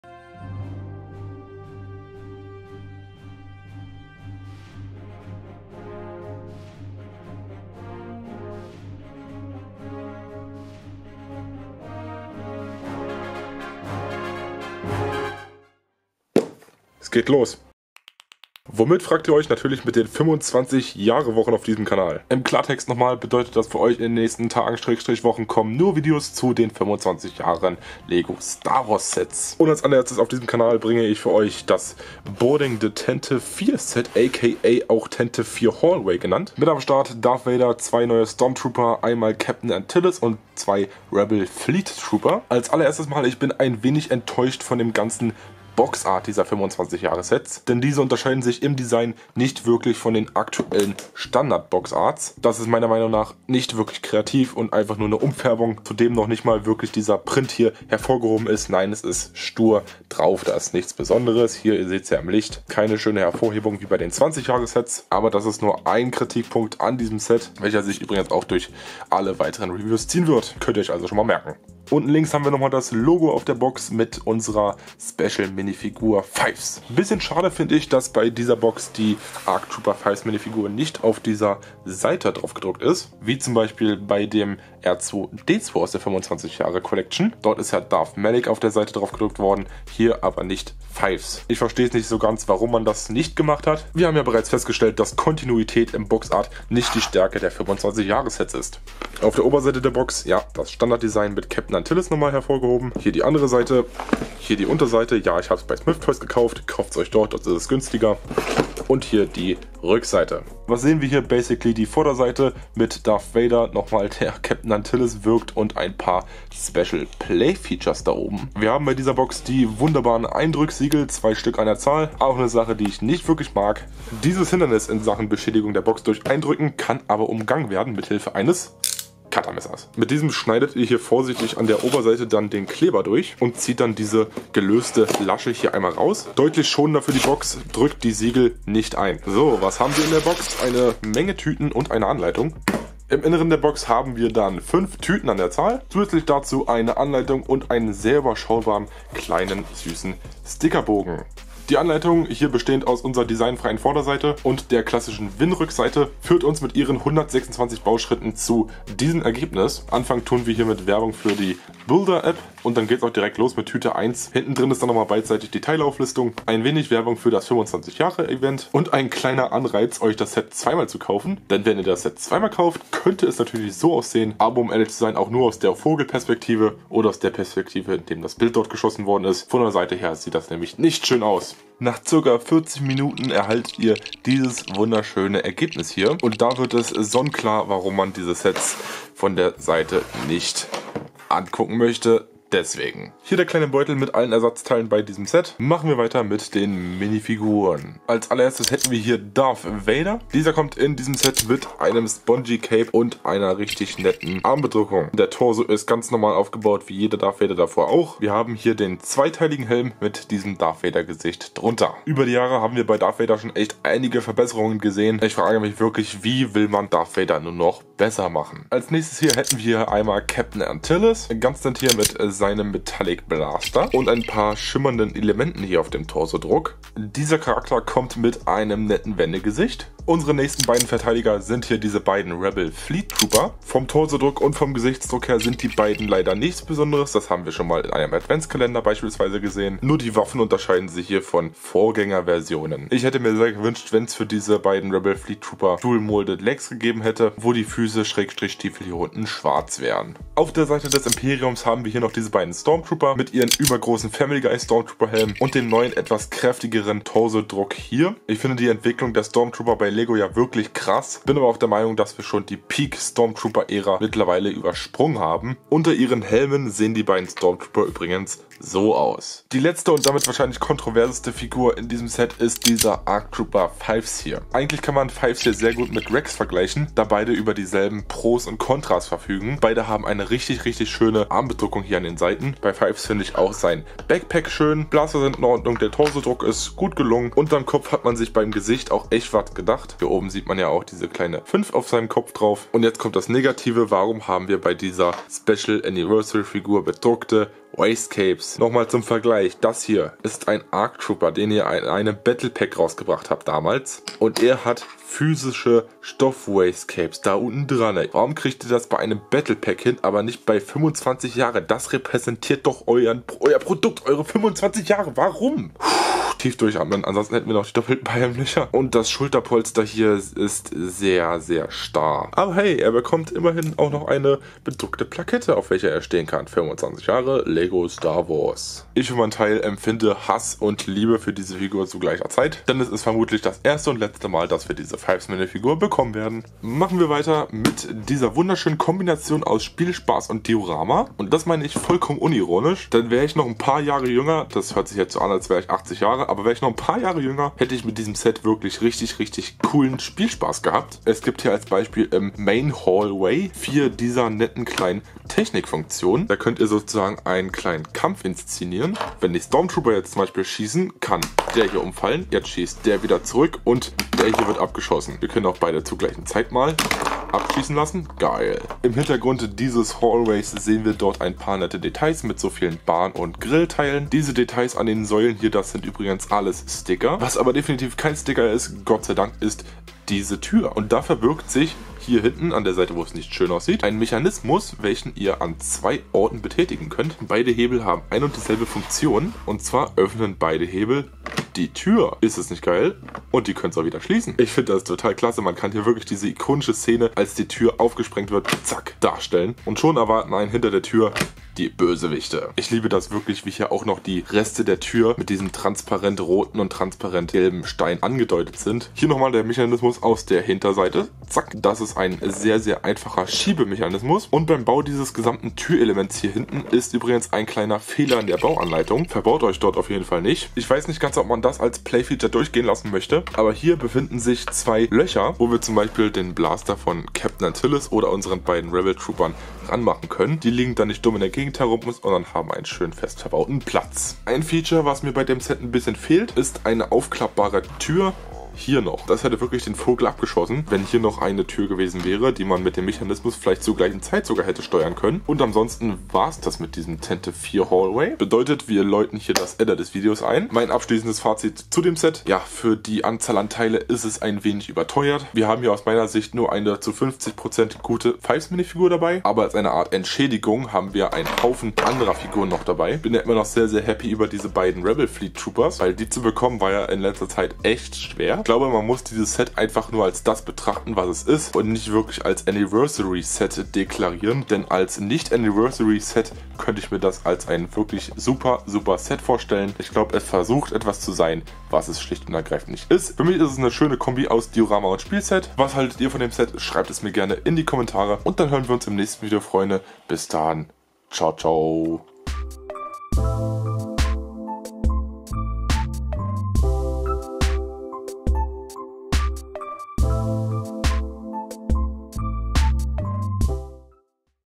🤔Gehts ohne FIVES? / LEGO STAR WARS Review / 75387 Boarding The Tantive IV - 25th Anniversary 👍
Hi, in diesem Video beleuchten wir das LEGO STAR WARS Set 75387 (Boarding The Tantive IV - 25th Anniversary Edition) einmal genauer. Wenn ihr euch überlegt dieses Set zu kaufen, schaut euch am besten dieses Review zum Set an und entscheidet dann.👌
💻 Sozial unterwegs:

⏱ Kapitel:

00:00 - 00:40 Intro
00:41 - 02:25 Design der Box und Unboxing
02:26 - 03:00 Anleitung
03:01 - 05:36 Minifiguren
05:37 - 07:25 Das Set
07:26 - 08:24 Fazit und Outro
👀 Auf der Suche nach speziellen Steinen oder Figuren? Schaut hier vorbei:

📑 Infos:

Niemand hat die Absicht Werbung zu machen! Dieser Kanal wird nicht von LEGO bezahlt oder beworben.